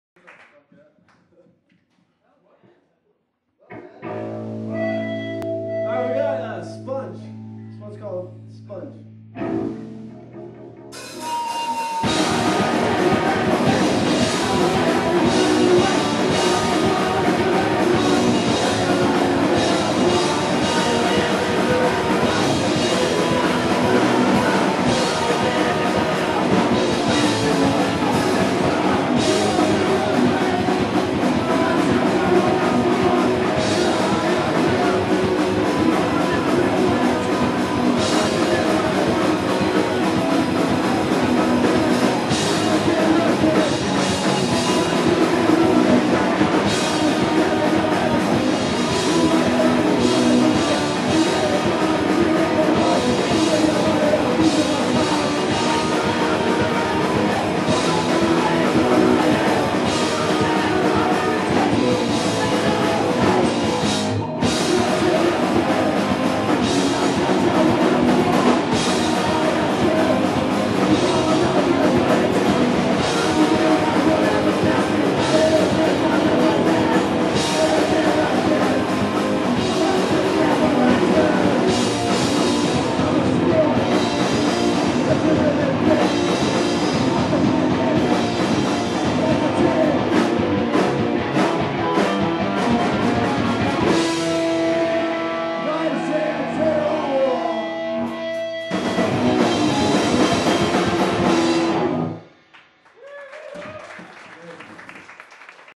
Violation (Santa Rosa, CA)(4/9)
In Chico California on the day of October the 5th in this Year Of No Lord 1012, at Monstros Pizza.